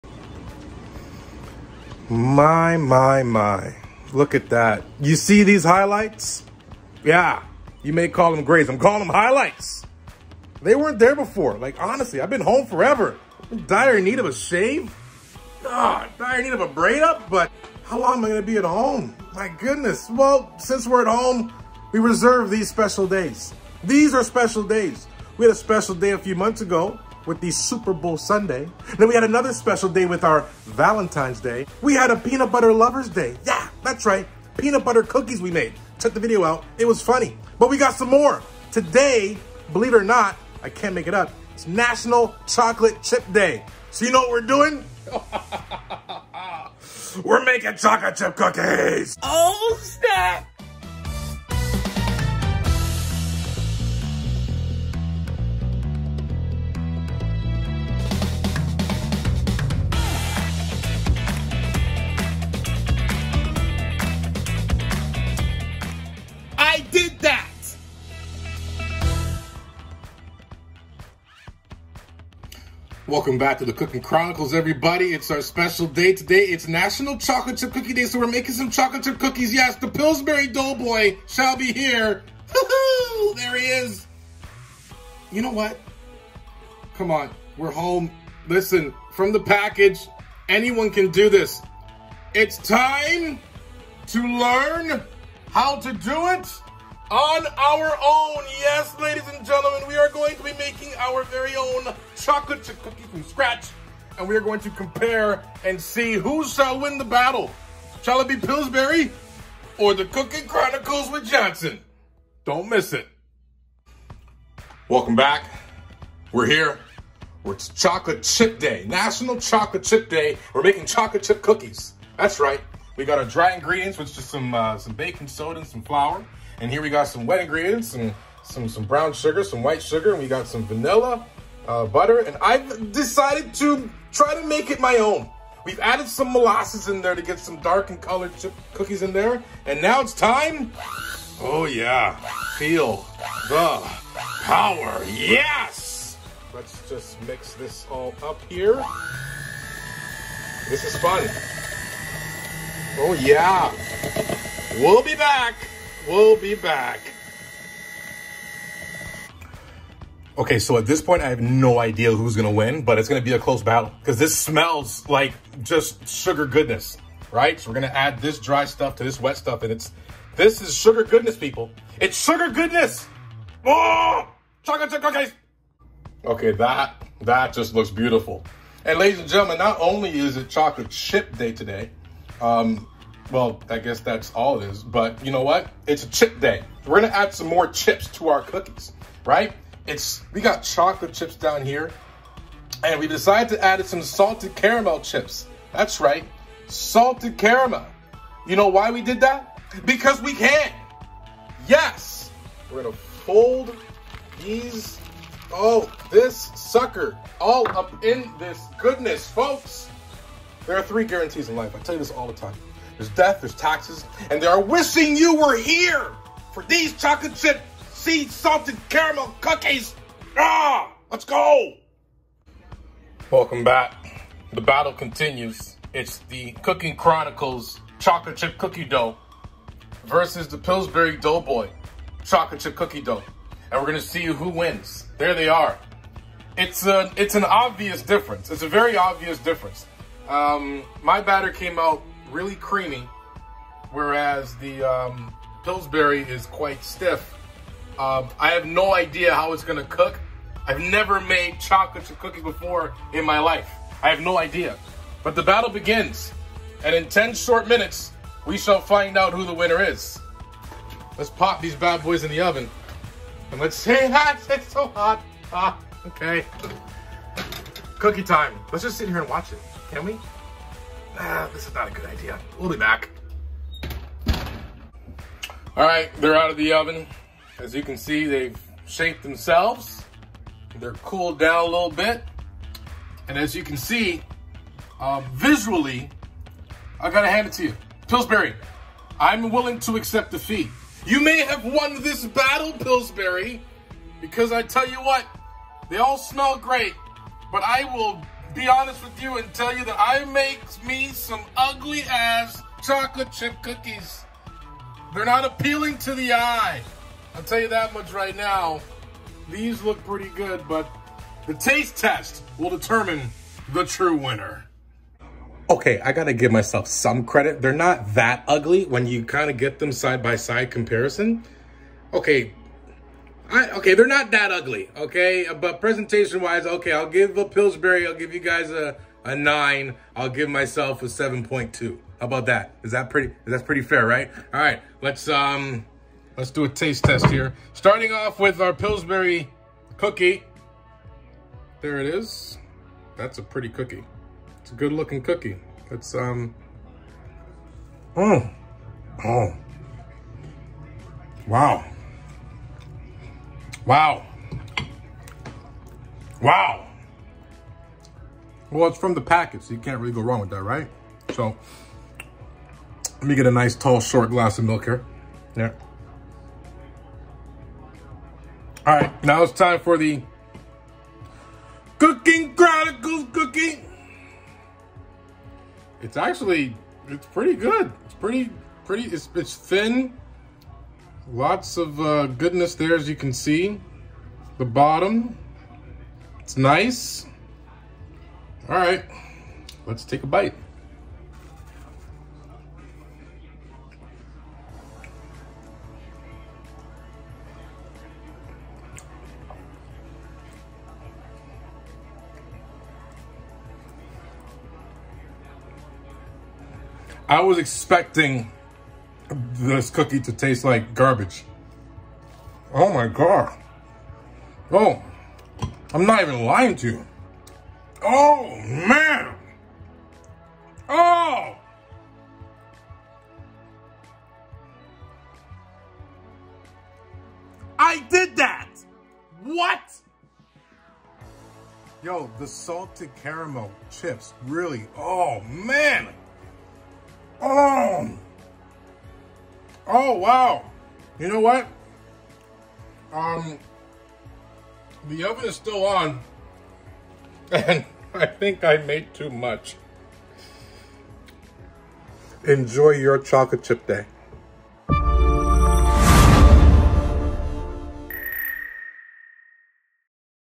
National Chocolate Chip Day: Chronicles with Johnson - Ep. 33
It’s NationalChocolateChipDay and it’s Pilsbury vs the Cooking Chronicles for cookie supremacy Click here for more videos

This is not an instructional video. Just me exploring good food and trying to make it taste good.

Ingredients:
Flour
Baking soda
Brown sugar
White sugar
Margarine
Vanilla extract
Molasses
Chocolate chips
Salted caramel chips